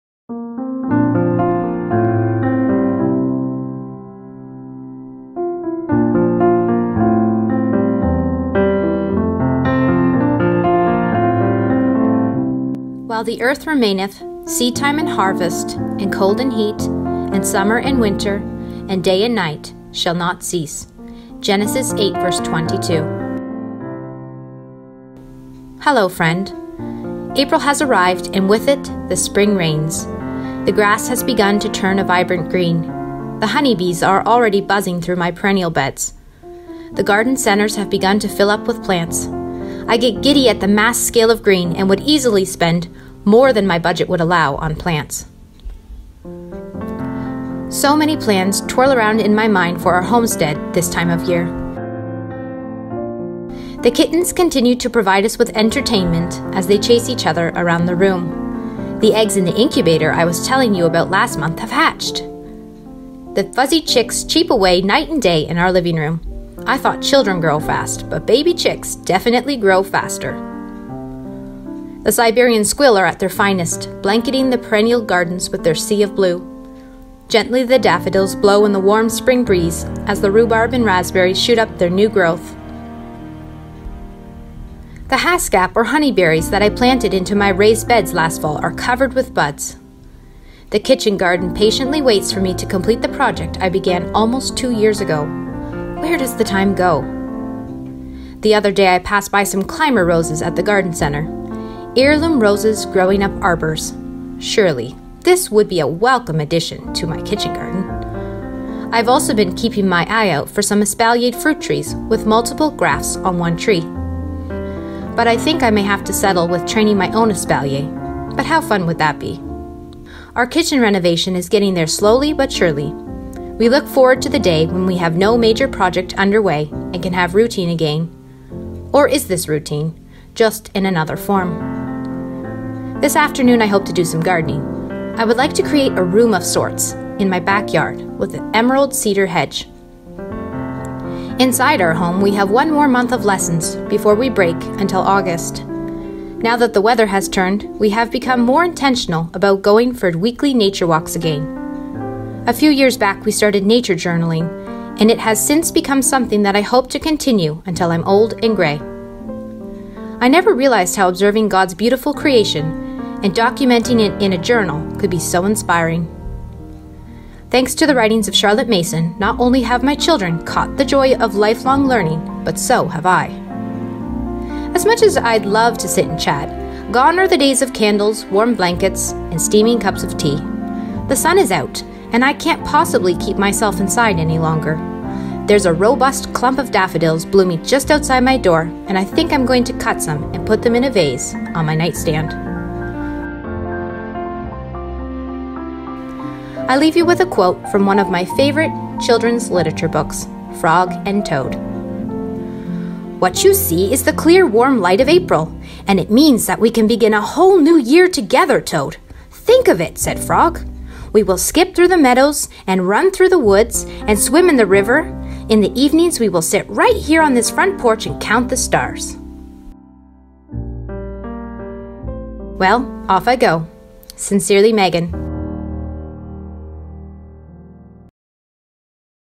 Letters from the Homestead Episode 2 || Slow Living | Slow Living Vlog | Copper Maran Chickens
Letters from the Homestead | April Journal | Letter 2

While the earth remaineth, seedtime and harvest, and cold and heat, and summer and winter, and day and night shall not cease." Genesis 8:22

Timestamps:
00:00- Intro
00:13 - Bible Verse
00:25 - Letter greeting
00:34- Honey bees
00:54- Kittens
1:00 - Baby chicks
1:13- Siberian squill flowers
1:18 - Daffodils
1:27 - Haskap berries
1:30- Kitchen garden
1:40 - Roses
2:15 - Gardening
2:29- Nature walks
2:34- Nature journaling
3:20- Quote from Frog and Toad
3:42- Outro

Hello friend,
April has arrived and with it the spring rains. The grass has begun to turn a vibrant green. The honey bees are already buzzing through my perennial beds. The garden centers have begun to fill up with plants. I get giddy at the mass scale of green and would easily spend more than my budget would allow on plants. So many plans twirl around in my mind for our homestead this time of year...